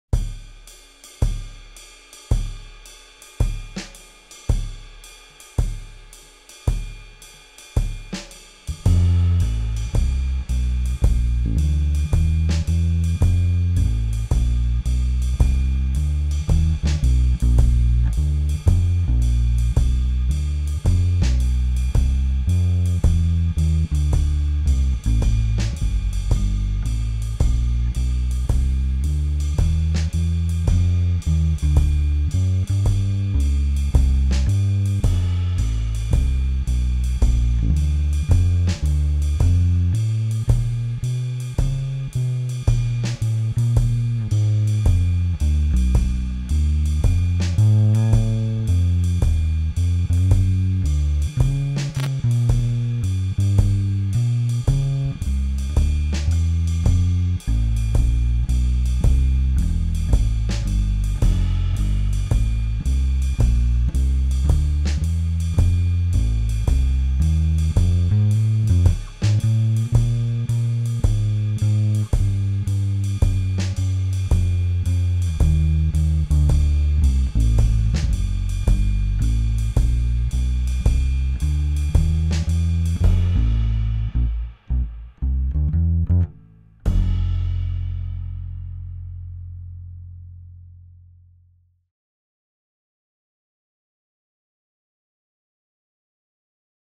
Takamine TB10 - [Bass Demo] - Walking the F Blues
A short bass demo of my Takamine TB10: just myself walking a few choruses of the F blues...
At some point, I received a WhatsApp message on my computer, and you can hear it come in!!!...  Ah ah ah...
In first position, my index finger is mostly sharp: oh well...